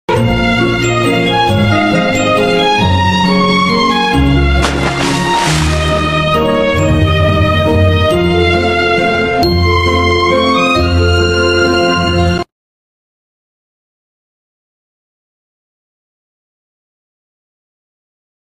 超惊艳！摄影师蹲守18小时拍到绶带鸟跳水！起跳、入水，随后又展翅高飞，画面超惊艳！｜【Hebei Moment河北：精彩时刻的记录者】
近日，河北平山。一只红色绶带鸟起跳、入水，随后又展翅高飞，画面超惊艳……摄影师: 绶带鸟个头小，飞行速度快，是最难拍的鸟之一，蹲了18个小时才拍到！来源：农民频道